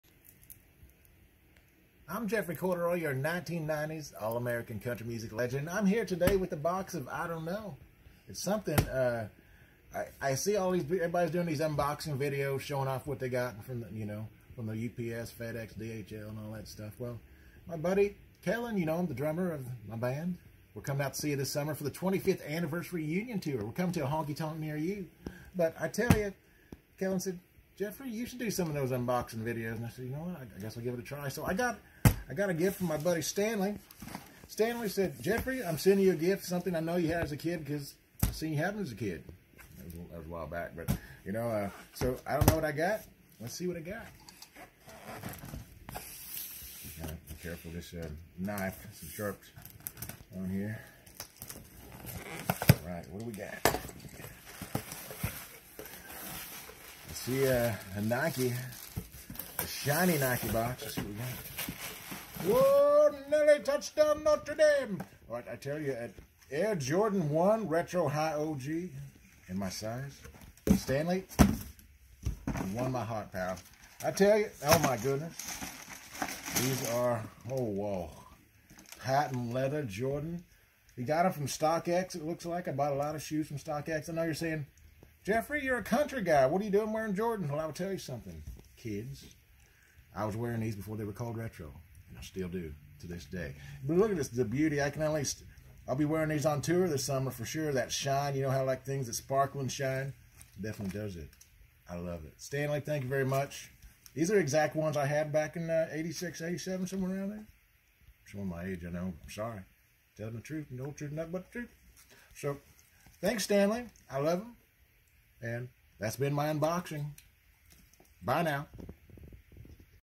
Review & Unboxing of Nike Jordan's Air Jordan 1 Retro OG BRED patent leather sneakers. Jordans
Unboxing of Nike Jordan's Air Jordan 1 Retro OG BRED patent leather sneakers.
Jeffrey wore these before they were considered retro.